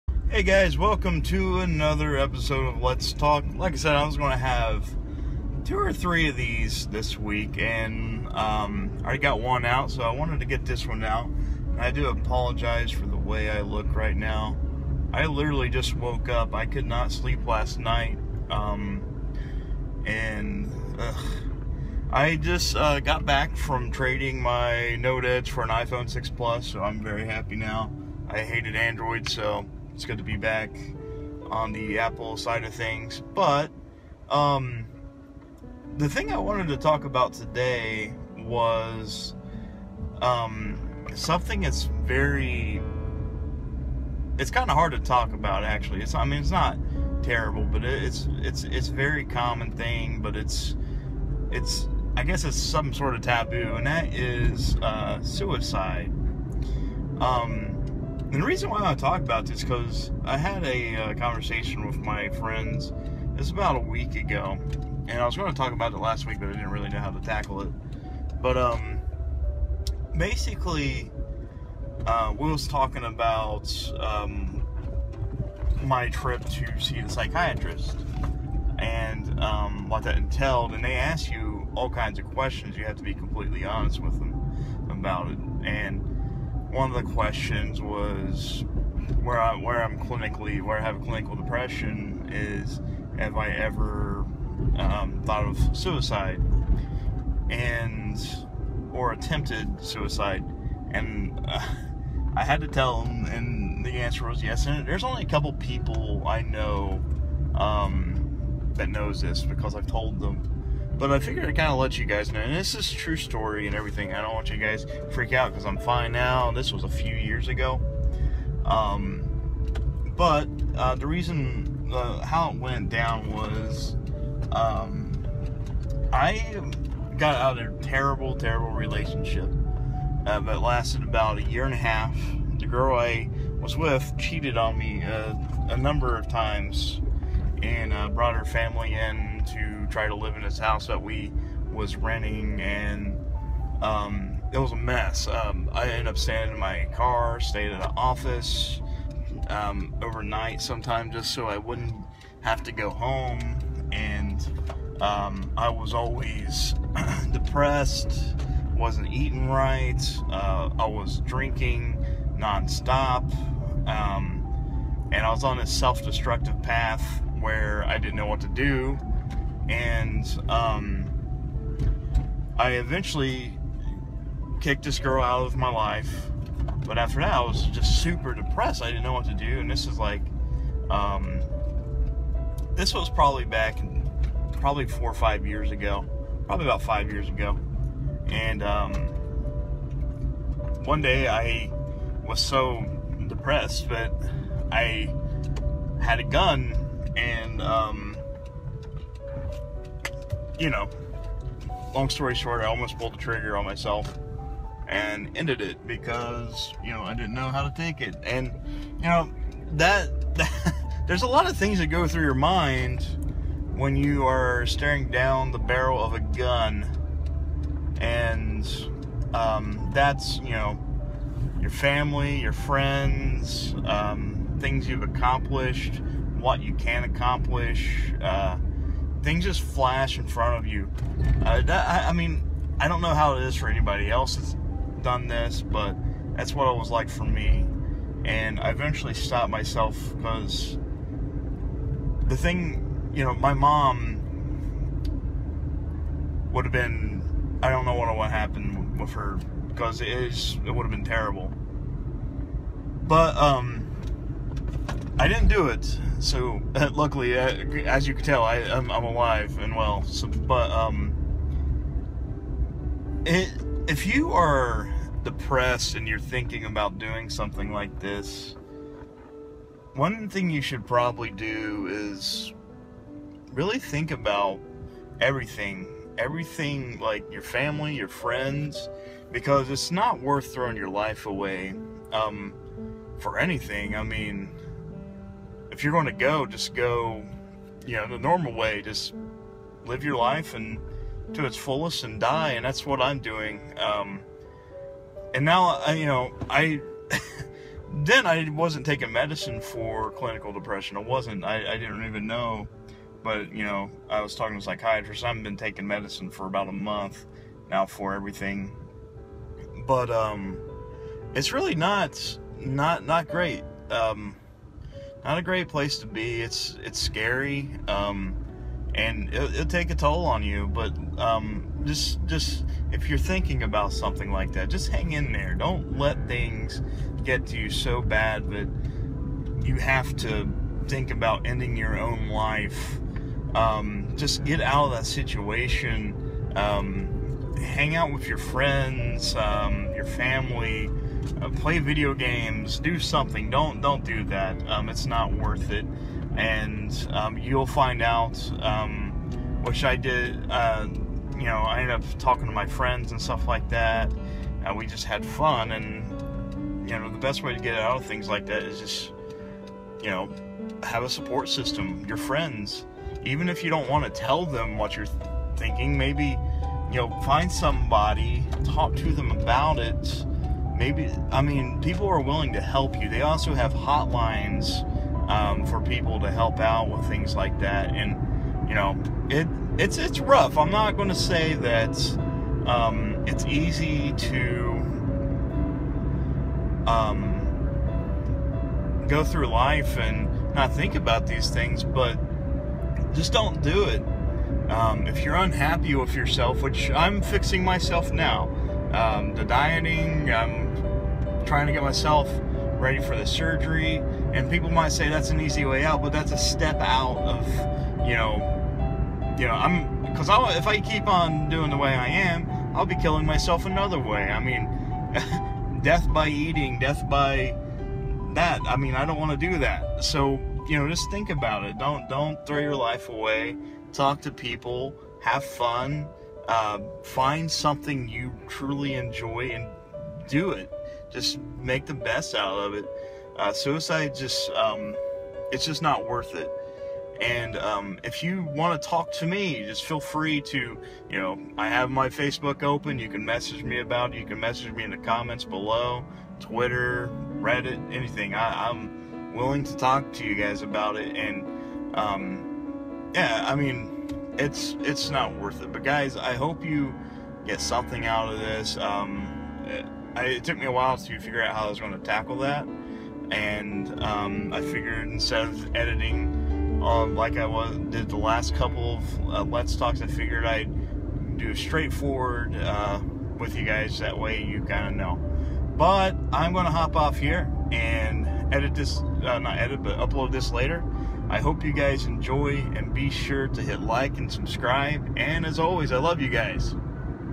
Let's Talk EP.10 About Suicide
For the second topic this week for Let's Talk I wanted to talk a little about suicide and why you shouldn't throw your life away.